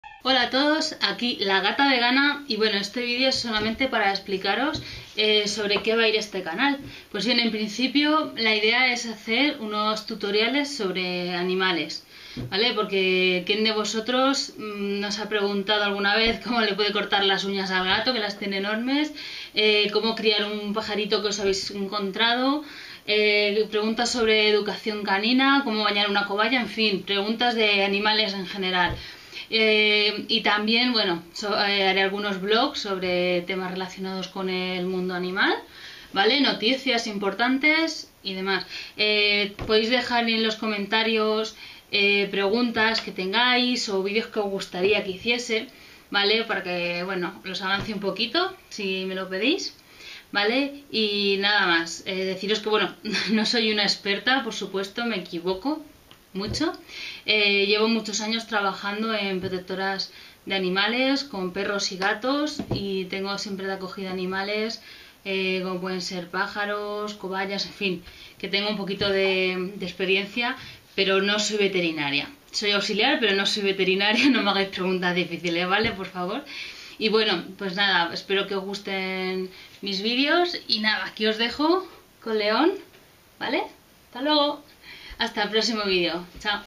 PRESENTACION DEL CANAL VLOG#1
Un resumen de lo que será este canal y que podréis encontrar en él.

Acuérdate de ponerlo en favoritos y compartirlo con tus amigos.

¡Gracias!  ;)